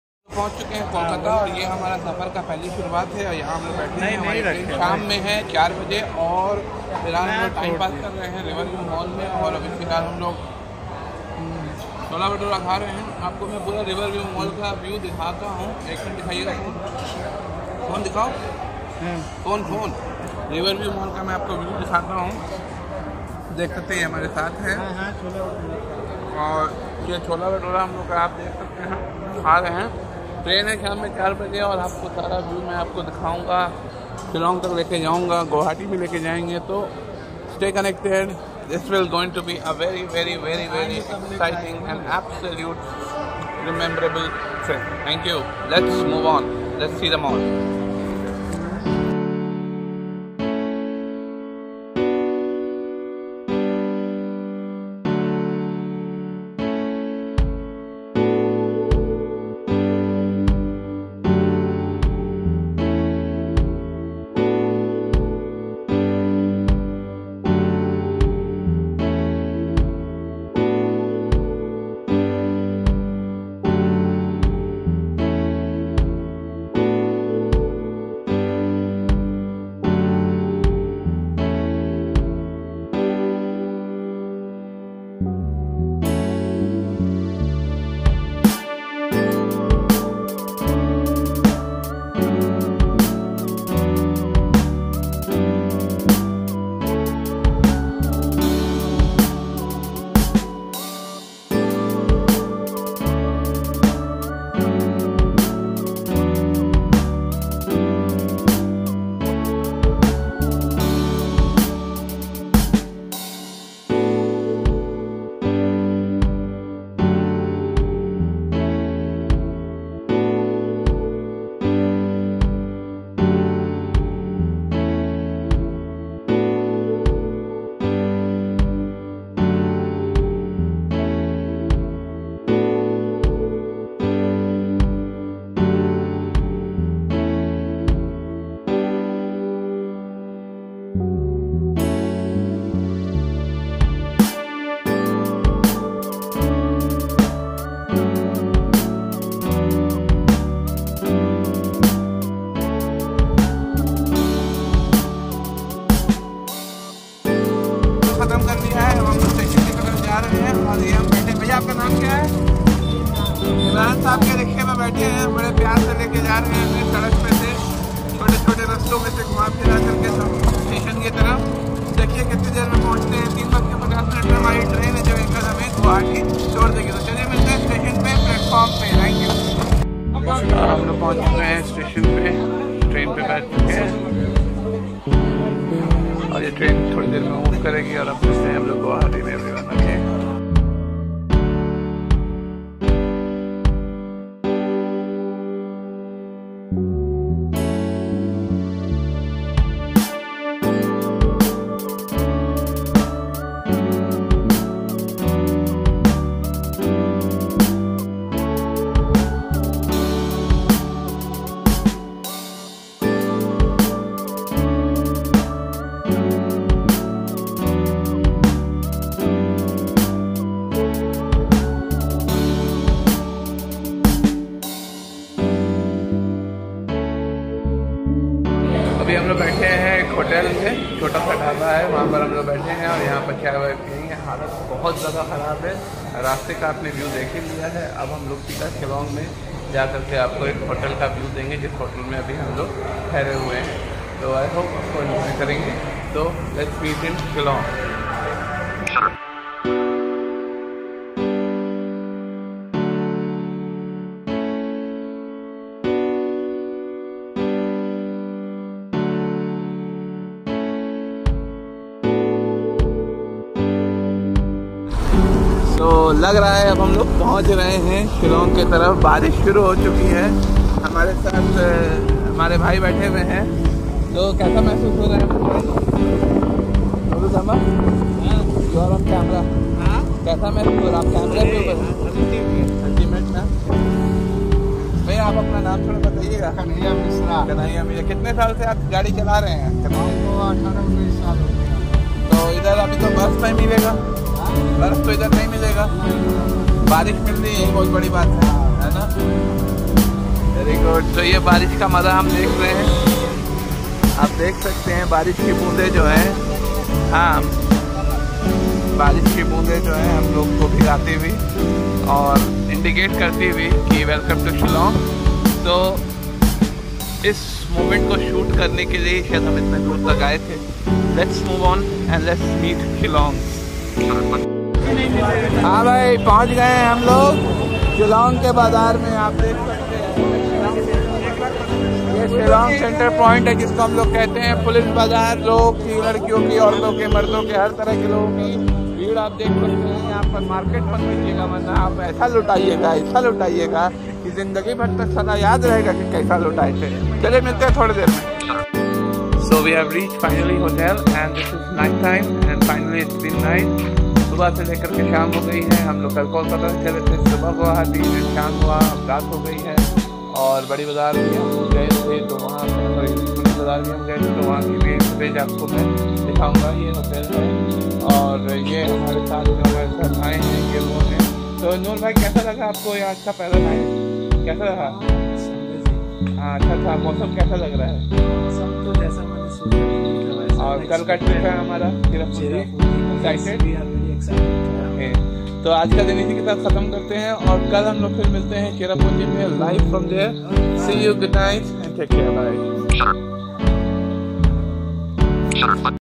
trip to Kolkata to shillong day1
in this video we will be going to Hill view Mall Kolkata from there will come back to howrah railway station and will take train to guwahati and from there will be going to shillong and this is first day of our trip.